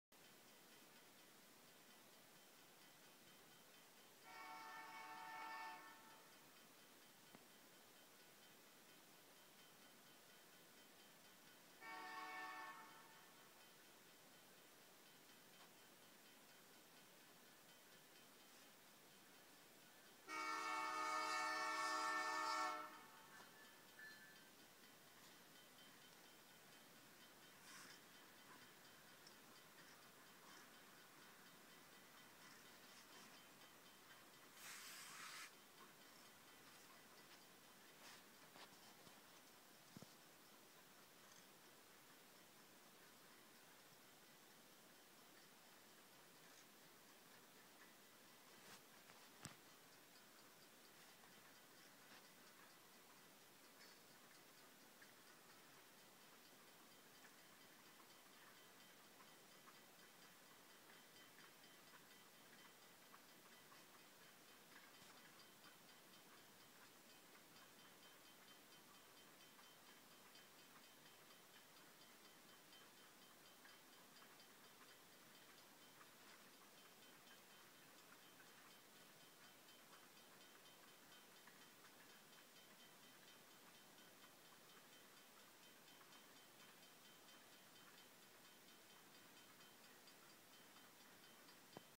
Action on the Dunnville spur
Welland yard local on the Dunnville spur with CP2250&2290